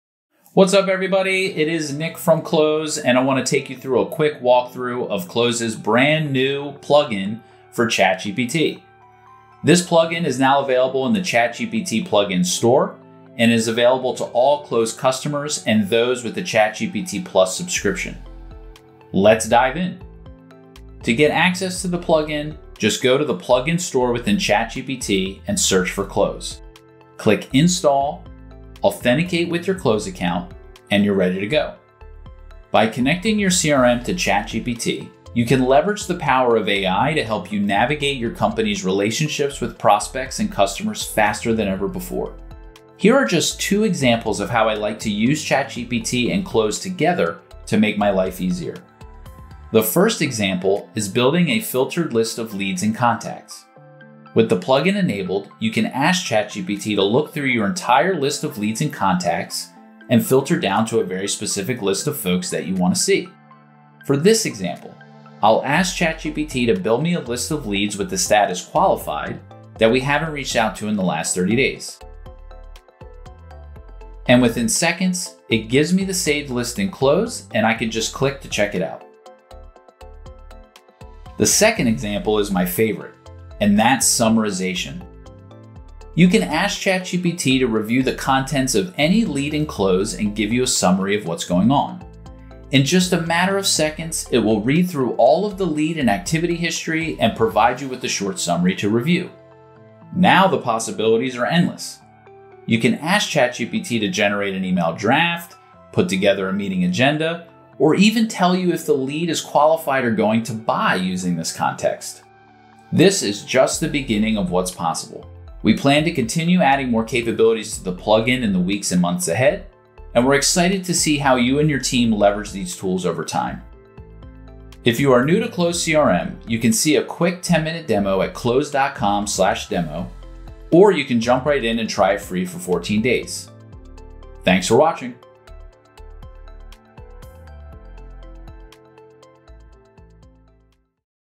Connect your CRM to ChatGPT with Close's plugin for ChatGPT!
Leverage the power of AI with Close’s plugin for ChatGPT, now available in beta to all Close customers and ChatGPT Plus subscribers.

Use Close’s ChatGPT plugin to save time building contact lists, summarize your relationship with leads and contacts, generate meeting agendas + follow up emails, and much more! See a demo of Close CRM and start your FREE 14-day trial